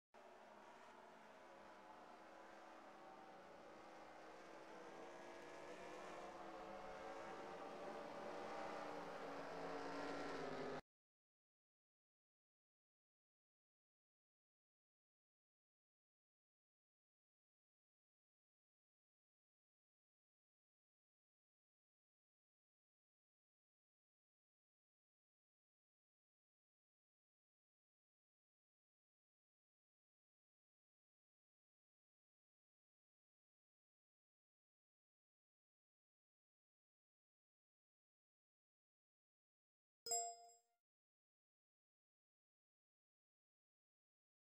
So I Get Black Flagged For This?
So I'm leading on the last lap in NORL at Watkins Glen. I'm (92) side-by-side with Brandon Schmidt (14) entering the bus stop and I had to run it wide to give him room. If I move closer to the left, we wreck. I come out of it with lead and am heading for home when I get black flagged by the league owner for cutting the course. I mowed the grass a couple times earlier in that race and got no penalty, plus others did the same and got no penalty. On top of that, it was even stated in the drivers meeting that the only way you can get a black flag for cutting the course is if the game assigns it to you. The admin gave it to me. It was also stated in the driver's meeting that you can use the grass, but you are responsible for what happens; I maintained control of my car. Do you guys think this was the right call or am I completely wrong???